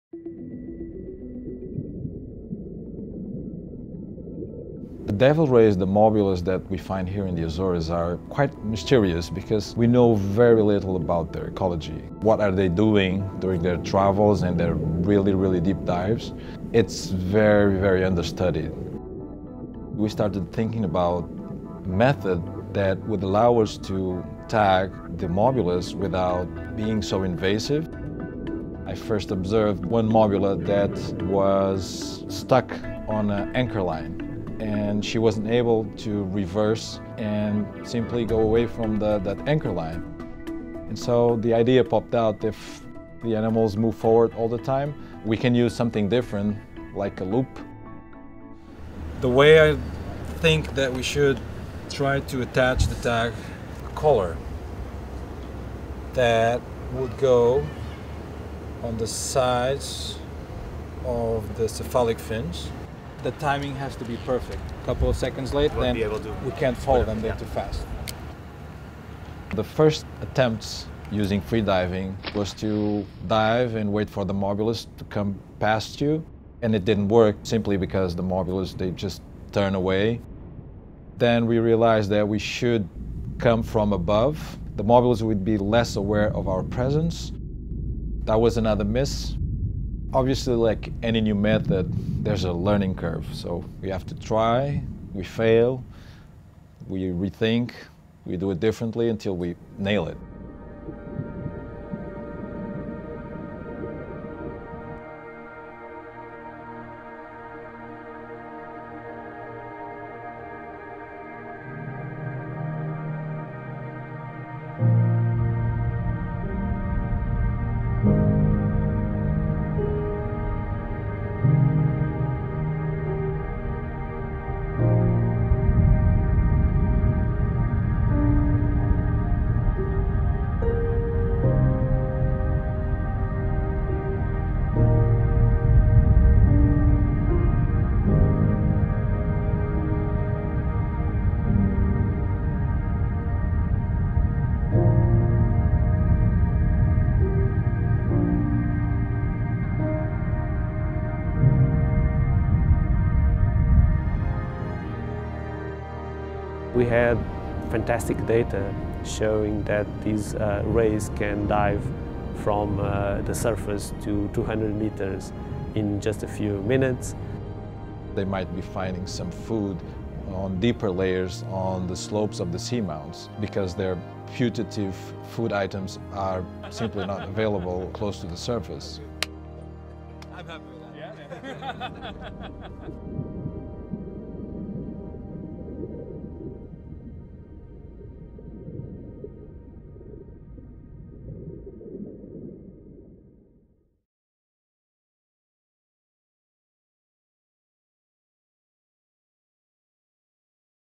Devil Ray tagging-Azores 2016 w/Portuguese Subtitles
Our expedition to the Azores with Dr. Emanuel Gonçalves in September of 2016 was one of our most amazing yet. A major feature was local scientist/free diver Jorge Fontes and his non-invasive technique for 'necklace' tagging the massive Mobula rays of the region. Watch how it was done!